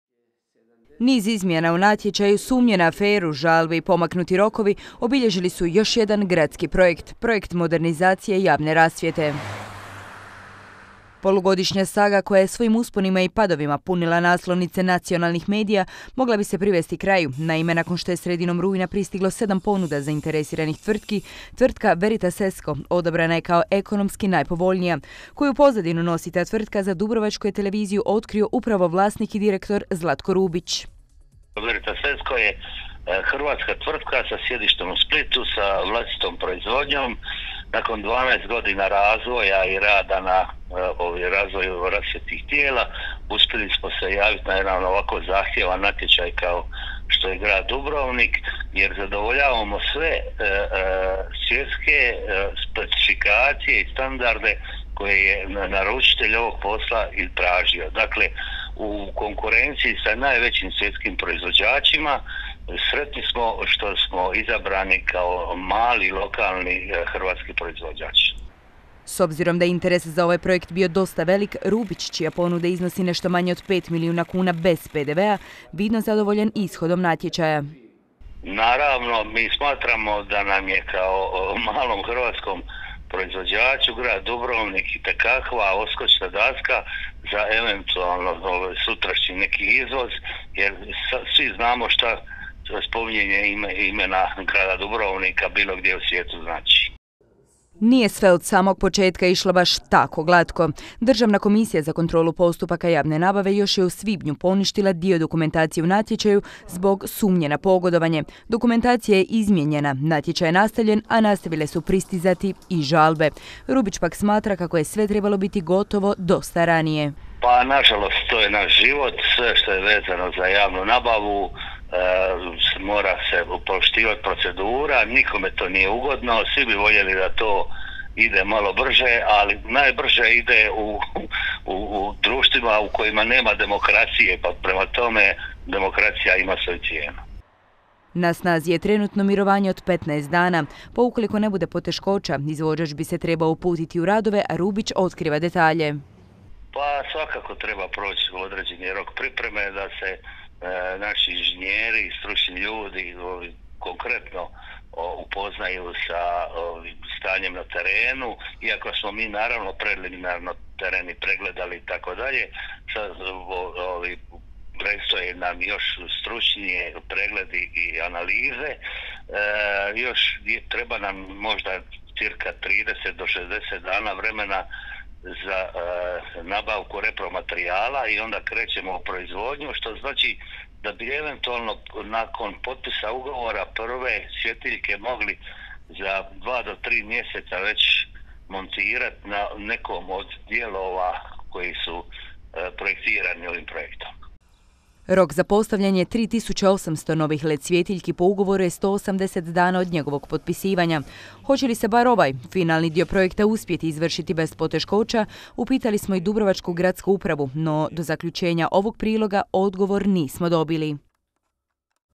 Odabran izvođač za modernizaciju javne rasvjete u Dubrovniku
DUTV Odabran je izvođač radova za projekt modernizacije javne rasvjete u Dubrovniku. Riječ je o tvrtki Veritas Esco iz Splita, a direktor Zlatko Rubić otkriva kako će s postavljanjem prvih svjetiljki krenuti sljedeće godine, ukoliko naravno ne bude žalbi na odabir ekonomski najpovoljnije ponude.